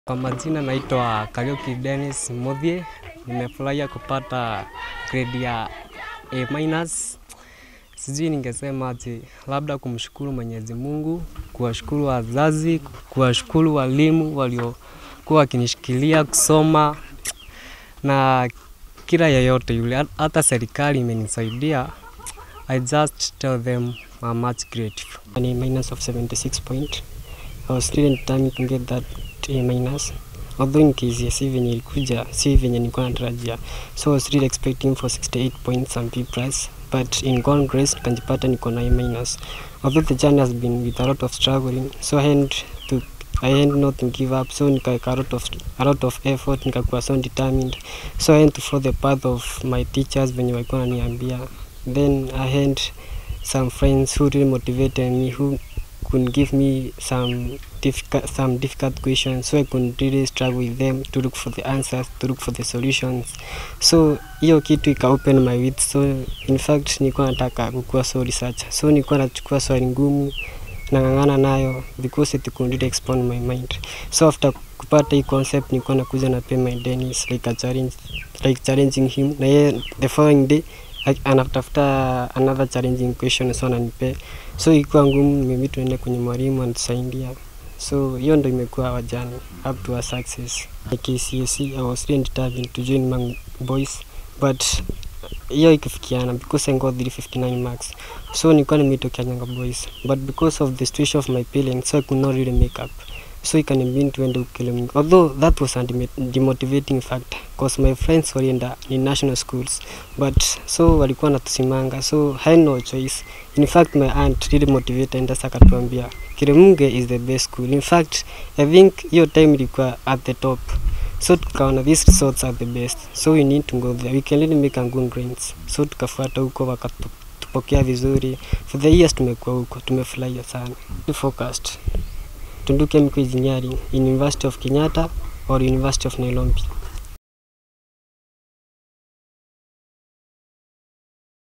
Kirinyaga identical twins score A- from a local day secondary school
Identical twins Davis Njoka and Dennis Muthii scored A minus in the just released examination